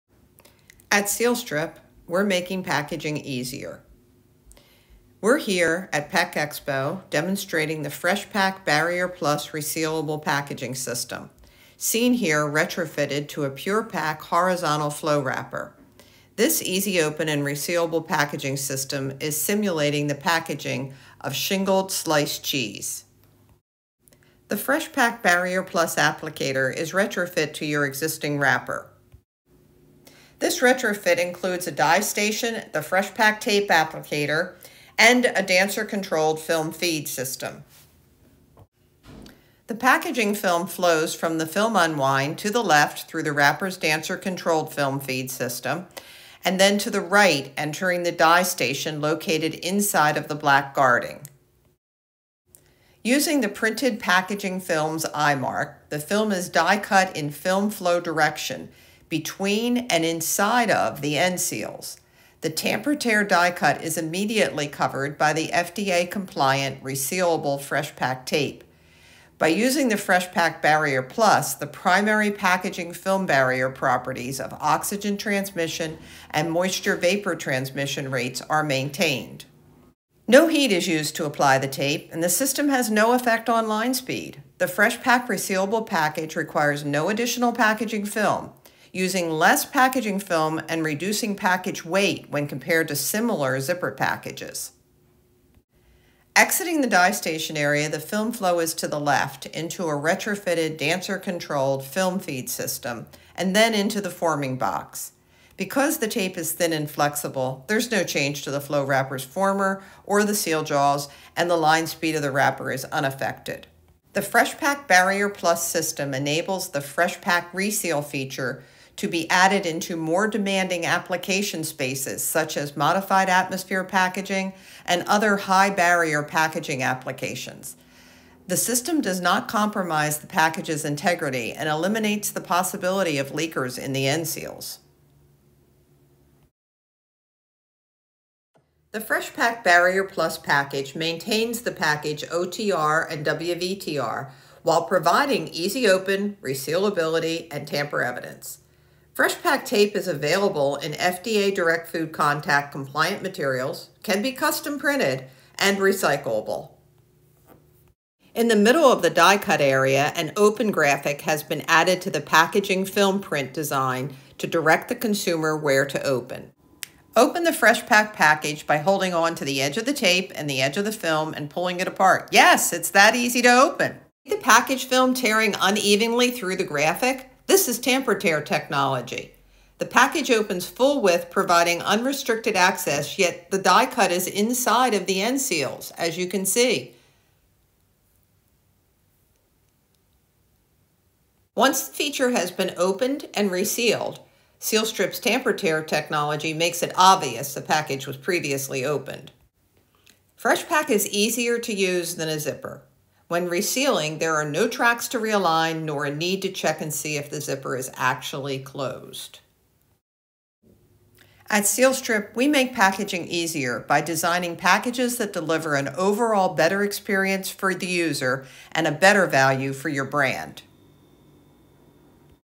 FreshPak BarrierPLUS at PackExpo 2021
At Sealstrip, we’re making packaging easier.
We’re here at PackExpo, demonstrating the FreshPak® BarrierPlus Resealable Packaging System, seen here retrofitted to a PurPak horizontal flow-wrapper. This easy open and resealable packaging system is simulating the packaging of shingled sliced cheese.
The FreshPak BarrierPlus applicator is retrofit to your existing wrapper. This retrofit includes of a die-station, the FreshPak tape applicator and a dancer controlled film feed system.
The packaging film flows from the film unwind, to the left through the wrapper’s dancer controlled film feed system, and then to the right, entering the die station located inside the black guarding.
Using the printed packaging films eye mark, the film is die cut in film flow direction, between and inside of the end seals. The TamperTear™ die-cut is immediately covered by the FDA compliant resealable FreshPak tape. By using FreshPak BarrierPlus, the primary packaging film barrier properties of oxygen transmission (OTR) and water vapor transmission rates (WVTR) are maintained.
No heat is used to apply the tape, and the system has NO effect on line speed. The FreshPak resealable package requires no additional packaging film, using less packaging film and reducing package weight when compared to similar zipper packages.
Exiting the die station area, the film flow is to the left, into a retrofitted dancer controlled film feed system, then into the forming box.
Because the tape is thin and flexible there is no change to the flow-wrappers former or the seal jaws, and the line speed of the wrapper is unaffected.
The FreshPak BarrierPlus system enables the FreshPak reseal feature to be added into more demanding application spaces such as modified atmosphere packaging (MAP) and other high barrier packaging applications. The system does not compromise the packages integrity and eliminates the possibility of leakers in the end seal.
The FreshPak BarrierPlus package maintains the package OTR and WVTR, while providing Easy Open, Resealability and Tamper Evidence. FreshPak tape is available in FDA direct food contact compliant materials, can be custom printed and recyclable.
 In the middle of the die cut area, an OPEN graphic has been added to the packaging film print design, to direct the consumer where to open.
Open the FreshPak package by holding onto the edge of the tape and the edge of the film and pulling apart. YES! It’s that easy to open.
See the package film tearing open, unevenly, right through the graphic? This is TamperTear technology!
The package opens full width providing unrestricted access to the product, yet the die cut ends inside the end seals, as you can see.
 Once the feature has been opened and resealed, Sealstrip’s TamperTear® technology makes it obvious the package was previously opened.
FreshPak is easier to use then a zipper. When resealing, there are no tracks to re-align nor a need to check if the zipper is actually closed.
To reseal the package, simply press the tape back against the package. Once resealed, the package contains the product and maintains freshness, while the TamperTear design provides visual evidence the package has been previously opened and resealed.